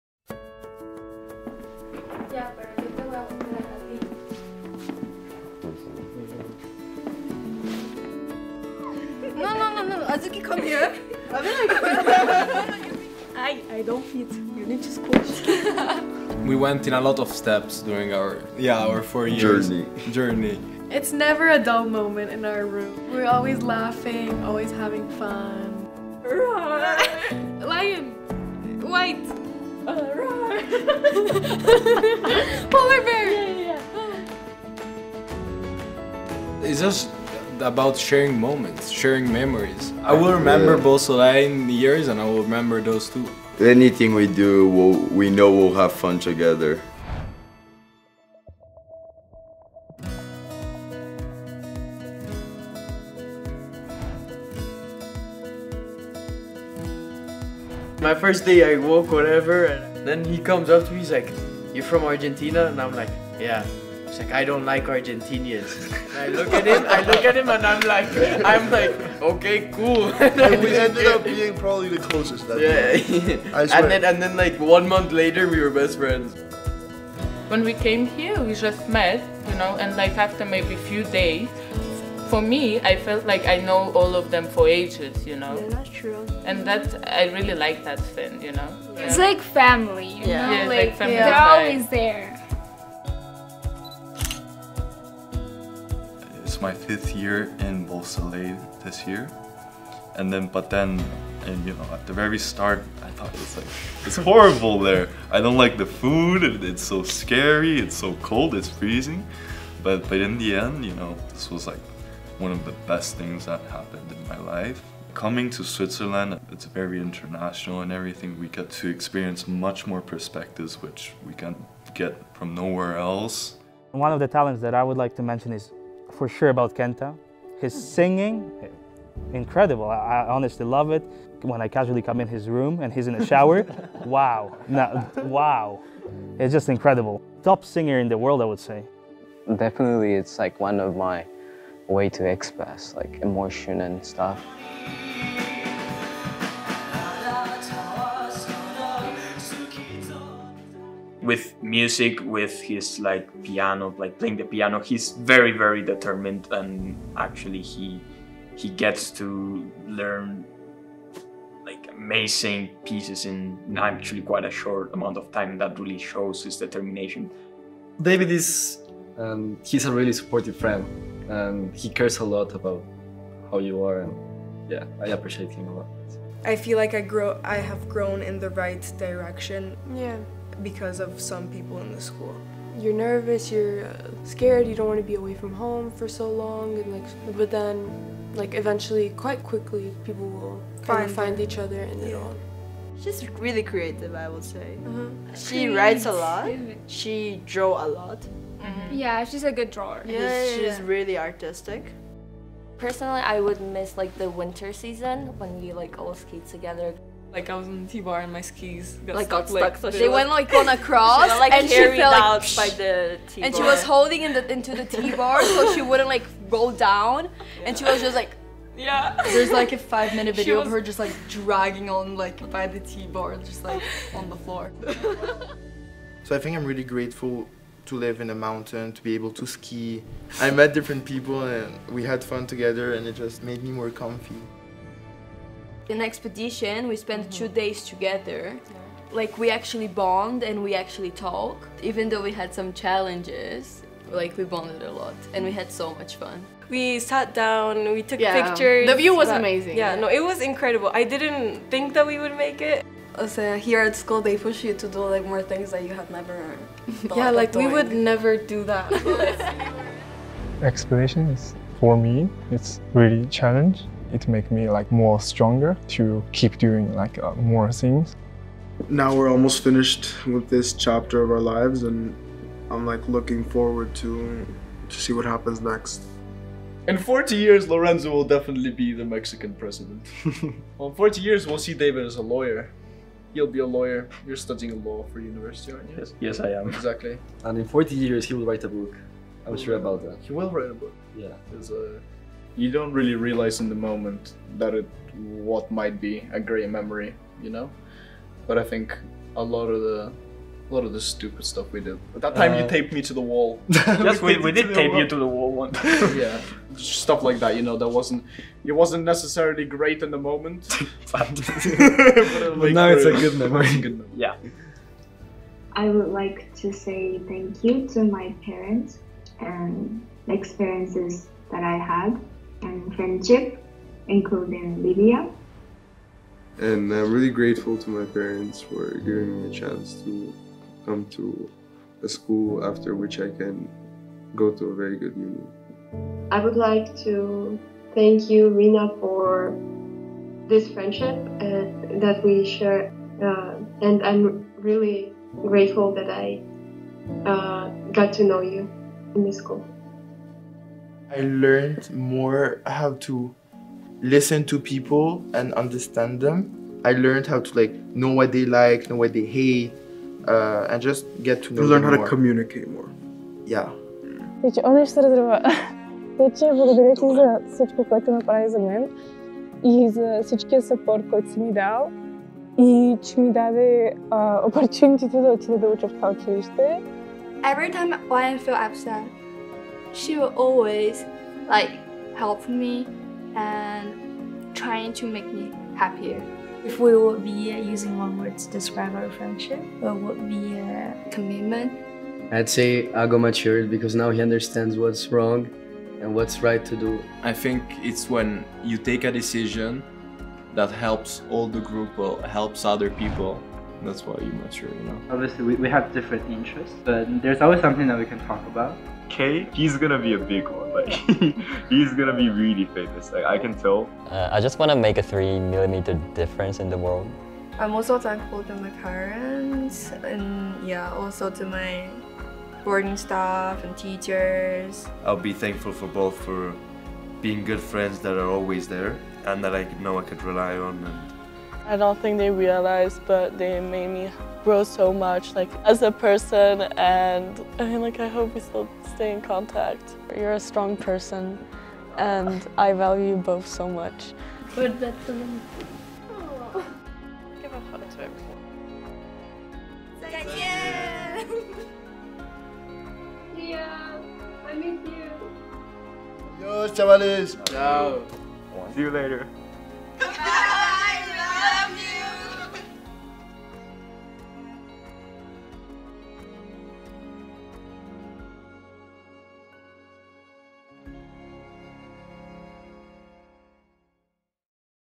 Beau Soleil Graduating Class 2022
"This was one of the best things that happened in my life."

Meet our Class of 2022 and discover why Beau Soleil is such a unique experience for our students.
Good memories, laughter and emotions are shared by our students while they reminisce about their Beau Soleil story.